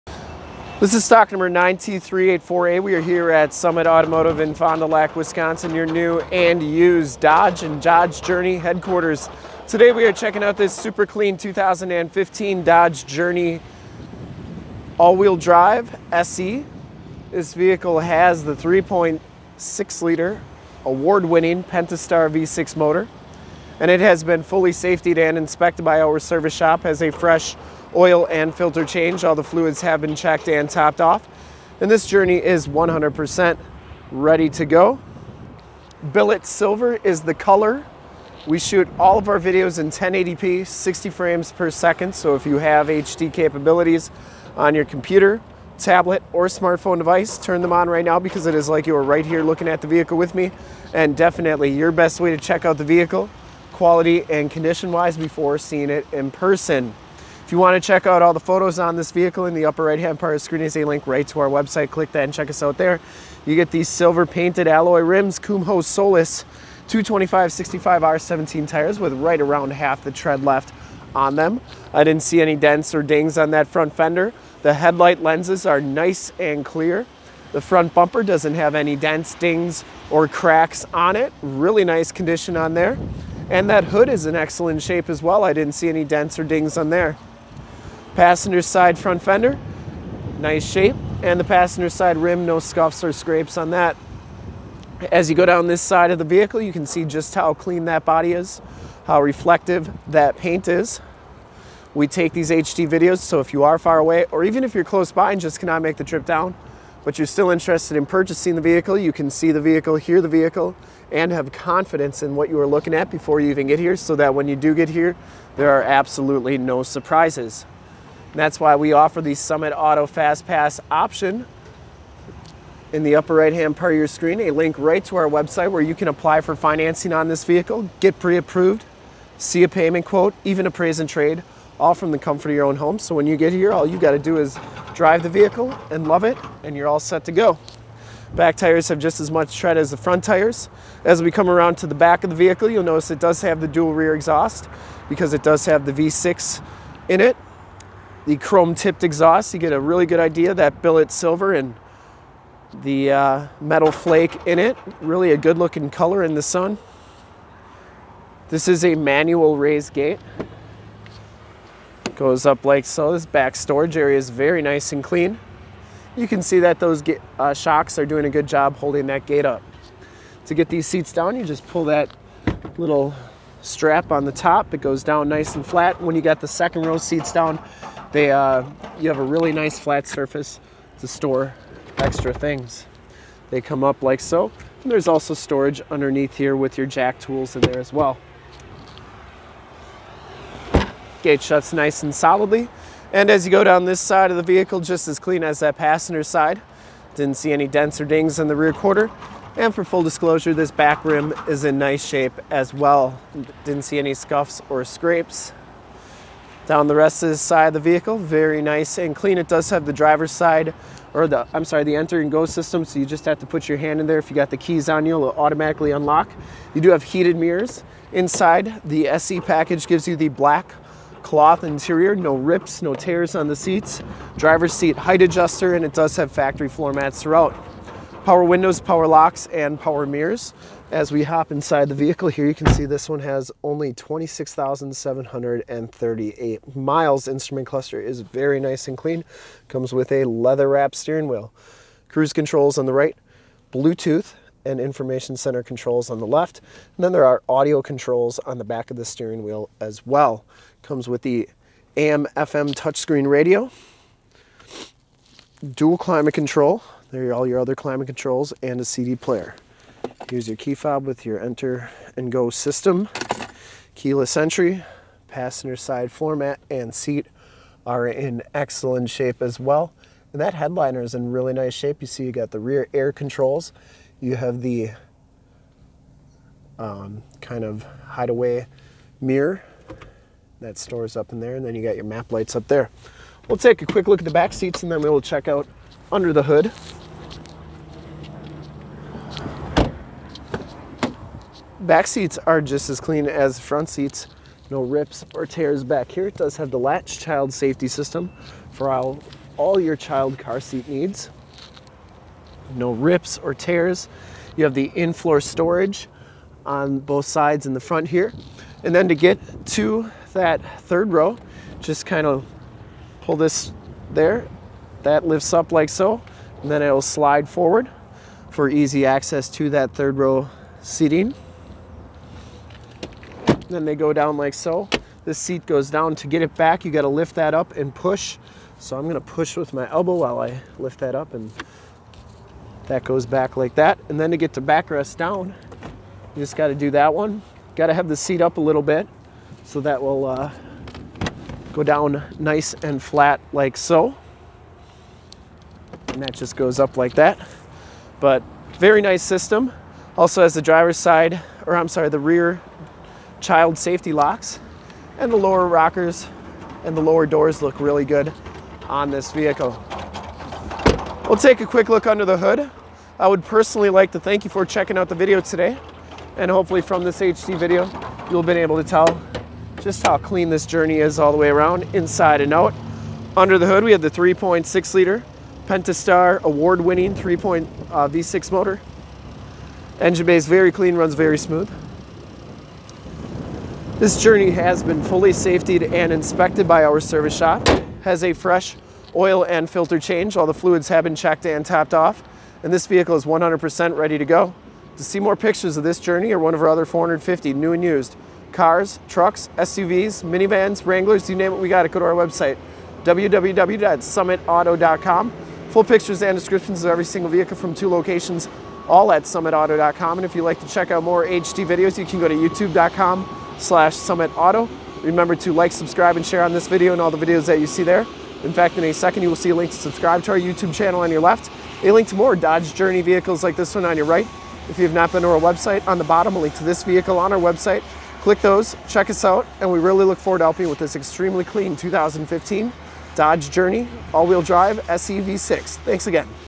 2015 DODGE JOURNEY V6 AWD 7 PASSENGER BILLET SILVER WALK AROUND REVIEW 9T384A SOLD! SUMMITAUTO.com
This 2015 DODGE JOURNEY V6 AWD 7 PASSENGER SE IN BILLET SILVER is the vehicle we did walk around review of today.

Thank you for checking out this video of this 2015 DODGE JOURNEY V6 AWD 7 PASSENGER SE IN BILLET SILVER

STOCK: 9T384A
PRICE: $14,999
MILES: 26,713
MAKE: DODGE
MODEL: JOURNEY
VIN: 3C4PDDAG3FT749571
LOCATION: FOND DU LAC OSHKOSH WISCONSIN, 54937 TRUCKS ON 41

1 OWNER! CLEAN TITLE HISTORY! 3.6 Liter DOHC Pentastar Engine, 283 Horsepower, SE Package, Automatic Transmission Center Console Shifter, All Wheel Drive Four Wheel Drive AWD, Dual Rear Exhaust, Non Smoker, Black Ebony Cloth Seats, Bucket Seats, 2nd Row Bench Seating, 3rd Row Seats Two Seater, Seven Passenger 7, Fold Flat Third Row Seats, Heated Power Mirrors, Electronic Stability Control Traction Control ESC, Kumho Solus KH16 P225/65 R17 Tires, Painted Alloy Rims Premium Wheels, Four Wheel Disc Brakes, Roof Rack Rails, Chrome Tipped Exhaust, AM / FM Radio Tuner, U Connect Hands Free Bluetooth Hands-Free Phone System Blue Tooth, Auxiliary MP3 Jack Portable Audio Connection, USB Jack Portable Audio Connection, Enter-N-Go System Keyless Entry System, Keyless Entry System, Push Button Start, Manual Raise Rear Gate, Rear Window Defroster, Adjustable Height Seatbelts, Driver and Passenger Front Air Bags, L.A.T.C.H. Child Safety System, Side Curtain Air Bags SRS Safety Restraint System, 2nd Row Power Windows, Multi-Function Steering Wheel Controls, Compass, Outside Temperature Display and Mileage Display, Dual Multi-Zone Climate Control , Fold Down Rear Seats, Rear Air Controls, Factory Floormats, Air Conditioning AC, Cruise Control, Power Locks, Power Windows, Automatic Headlights Autolamp, Tilt/Telescope Steering Wheel, Bright Silver Metallic, ONE OWNER! CLEAN AUTOCHECK! Very very clean inside and out! This is one of the sharpest 2015 Dodge Journey suvs we have ever had on our lot! Make your move before this super clean 4wd is gone!

STOCK: 9T384A
PRICE: $14,999
MILES: 26,713
MAKE: DODGE
MODEL: JOURNEY
VIN: 3C4PDDAG3FT749571